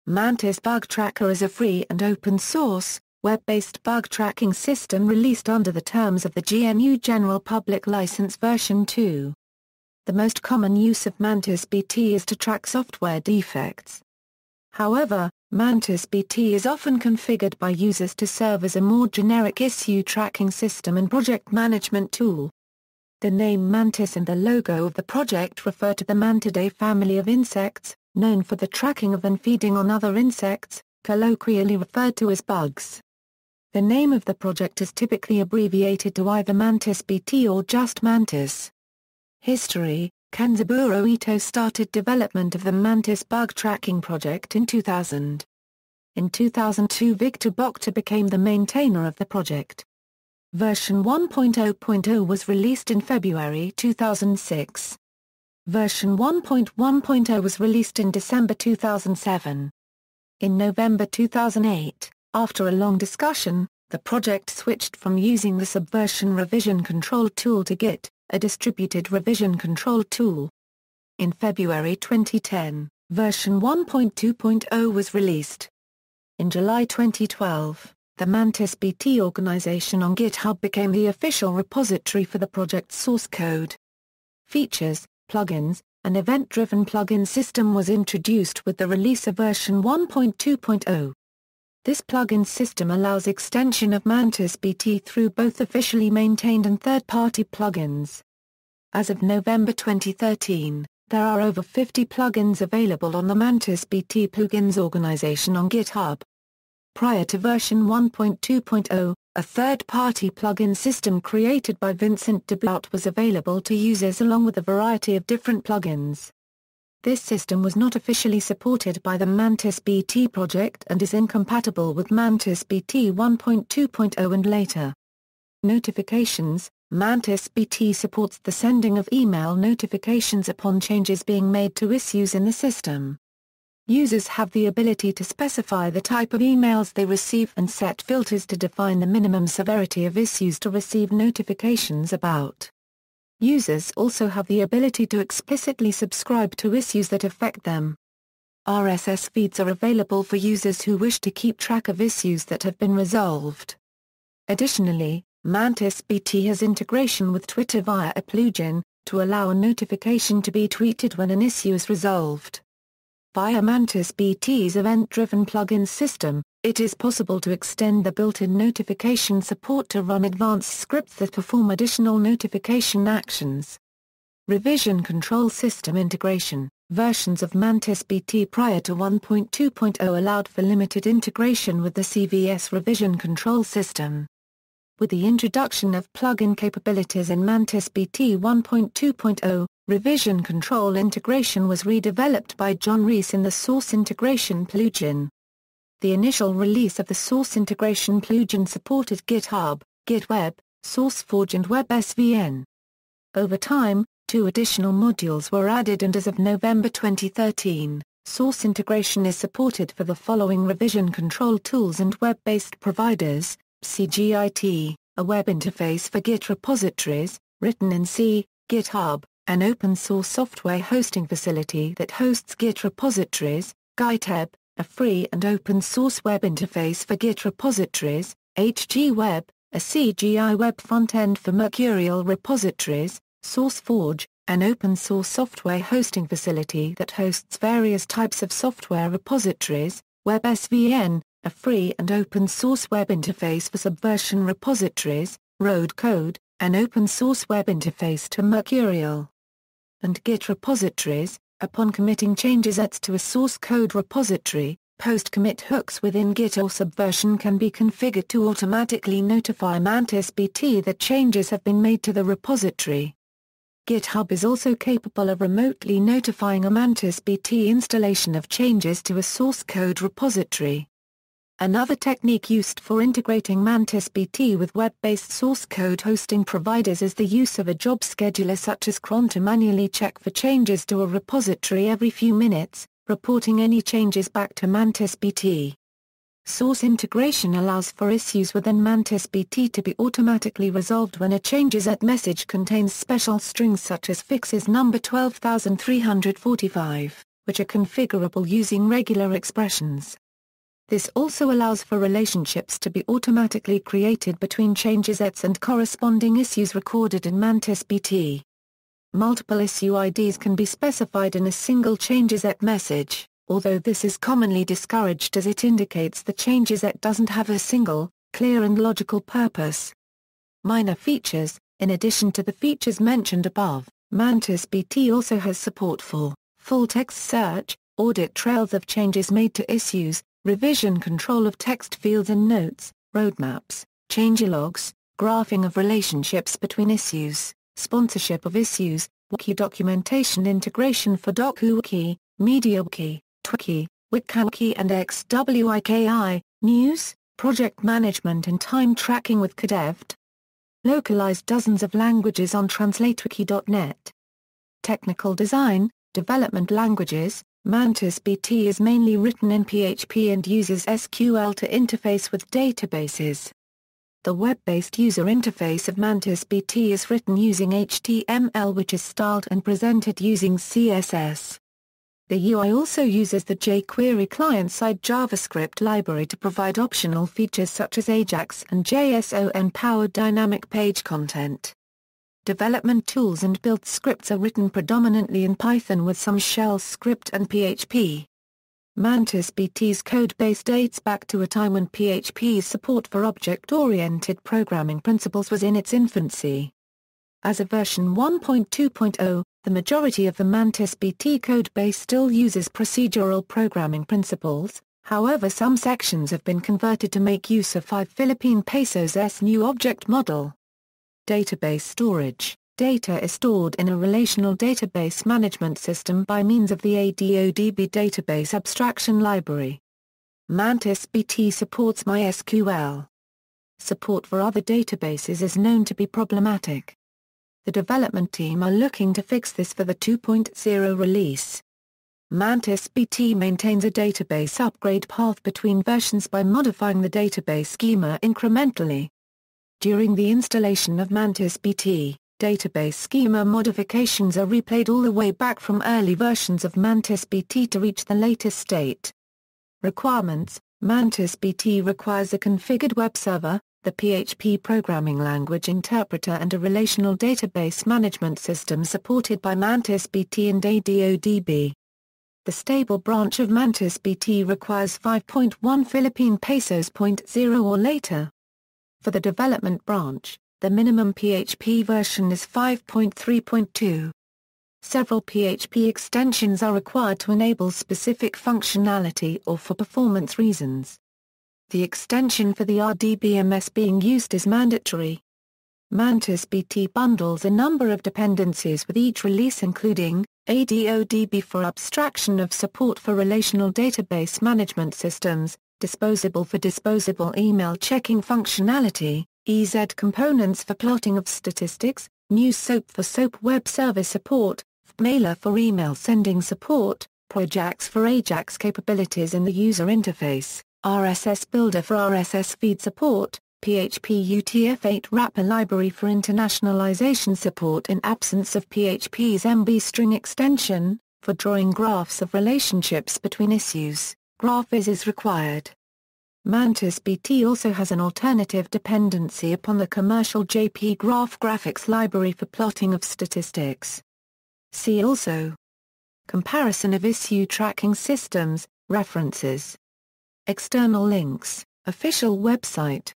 Mantis Bug Tracker
Mantis Bug Tracker is a free and open source, web-based bug tracking system released under the terms of the GNU General Public License version 2. The most common use of MantisBT is to track software defects. However, MantisBT is often configured by users to serve as a more generic issue tracking system and project management tool.
The name Mantis and the logo of the project refer to the Mantidae family of insects, known for the tracking of and feeding on other insects, colloquially referred to as "bugs". The name of the project is typically abbreviated to either MantisBT or just Mantis.